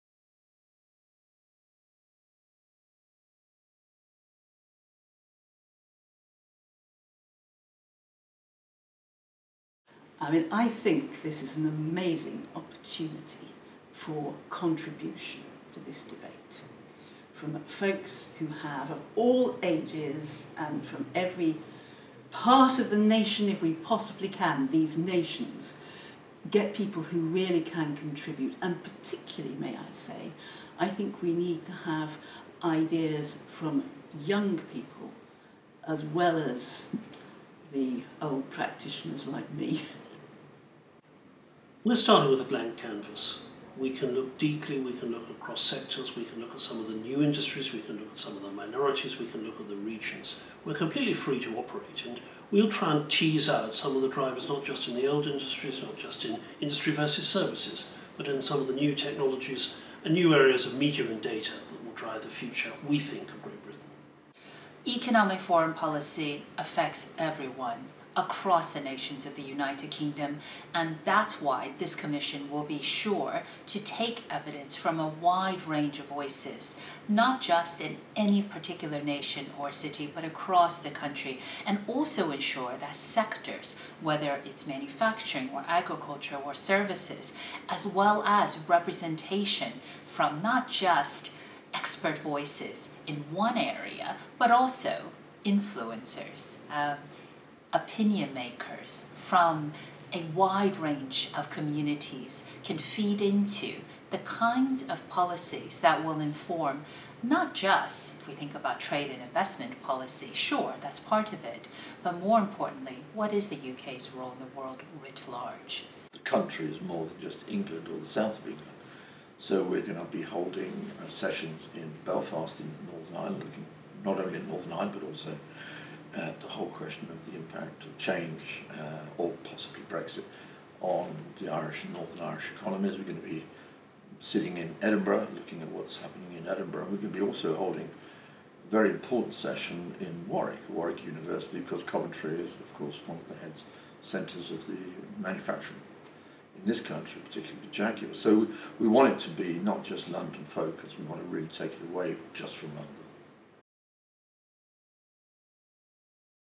How will the LSE Economic Diplomacy Commission ensure representation?
The LSE Economic Diplomacy Commission, a LSE IDEAS initiative, is a forum for informed strategic discussion and evaluation of the future of the UK’s economic diplomacy.

Susan Scholefield, Guy Monson, Linda Yueh and Michael Cox explain how the LSE Economic Diplomacy Commission, which launched in October 2019, will ensure representation during the hearings and evidence sessions.